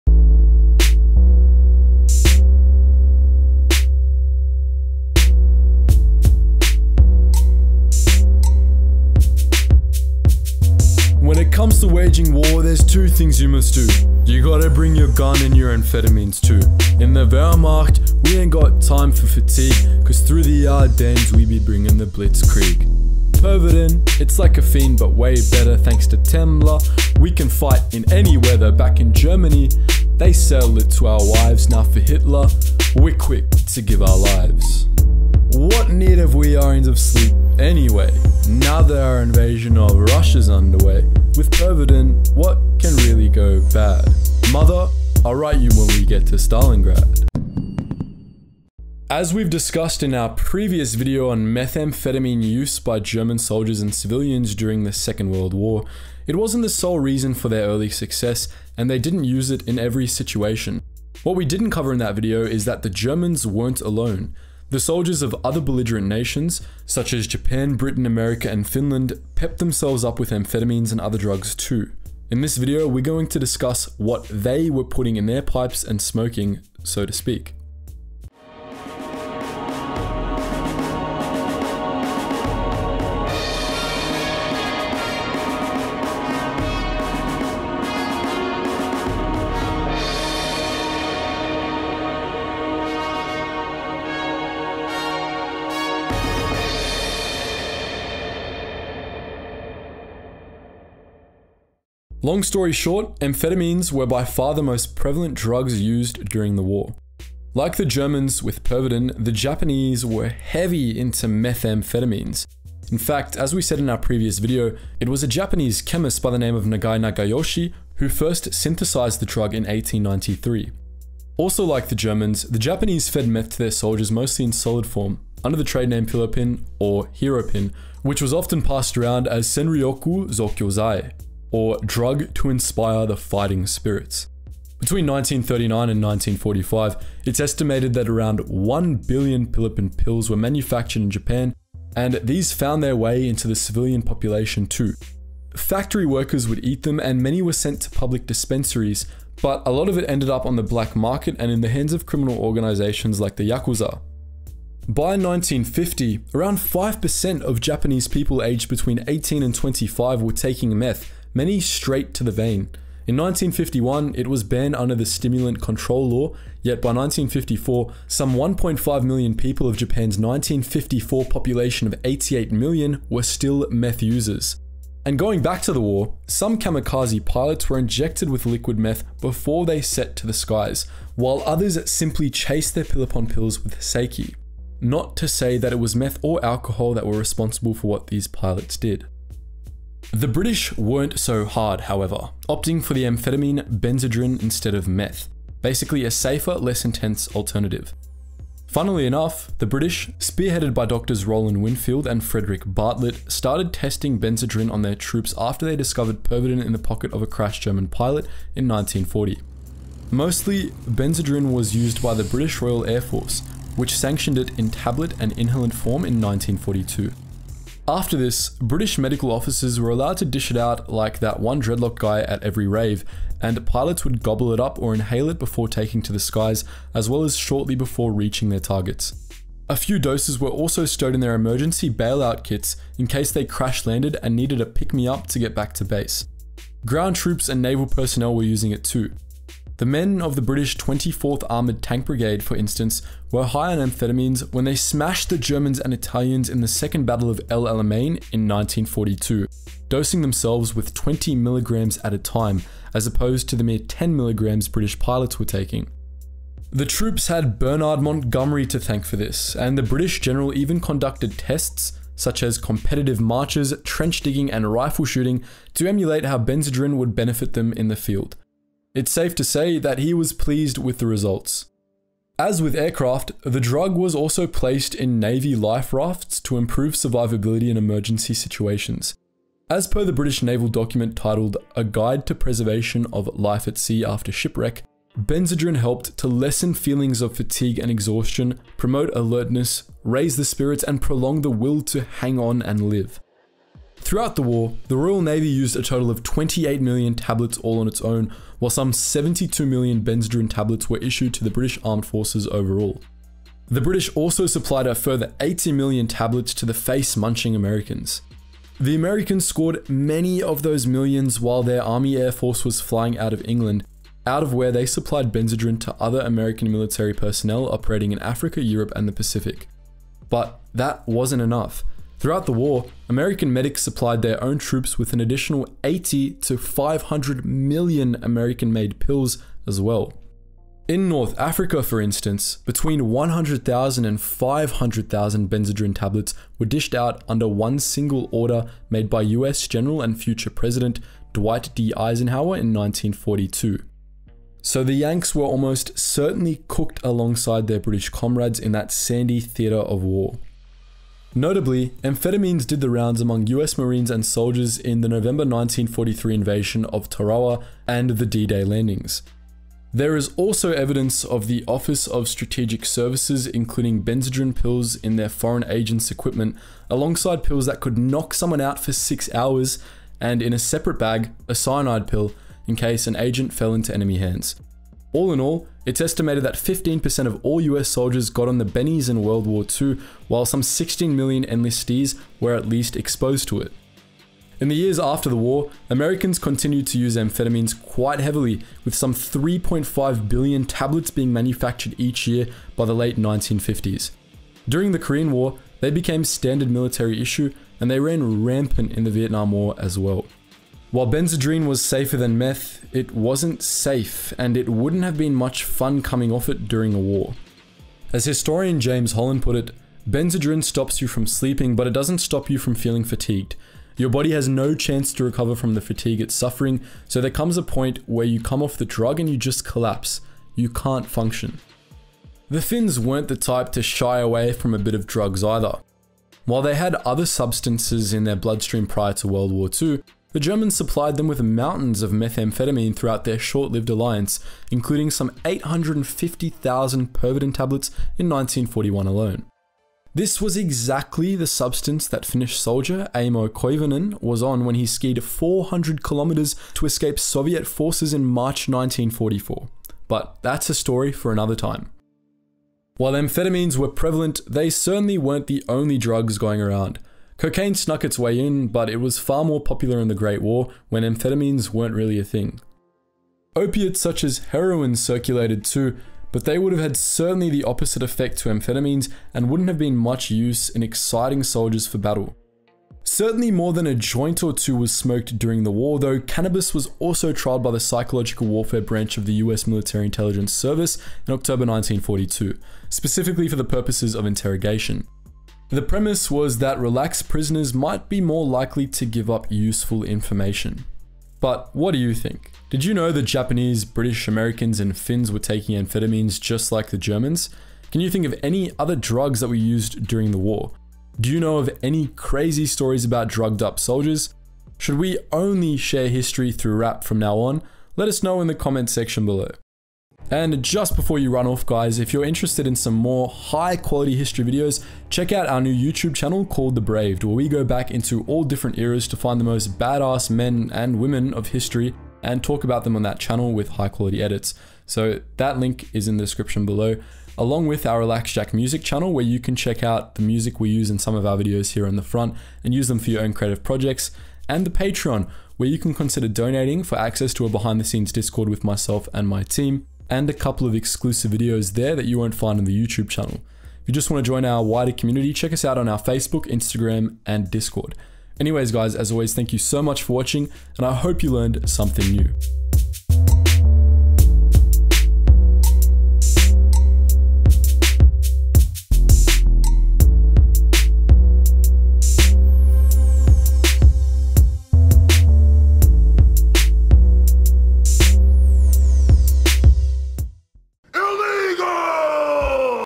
All (Now) Illegal Drugs Used by WW2 Fighting Armies
Most major armies in WW2 liked to spice it up a bit in WW2 by introducing stimulant drugs to their soldiers & military personnel.

🎬Video Credits:
Narrator - Cam
Editors - Giorgi, Steve
Writer - Nick, Brad Dare
Researcher - Daniel

Chapters

0:00 My Sick Beats
0:48 Introduction
1:47 Japan's Drug Use during World War 2
3:09 Allied Drug Use during World War 2
7:02 Finland's Drug Use during World War 2
7:32 Other Drugs used during World War 2
8:11 Conclusion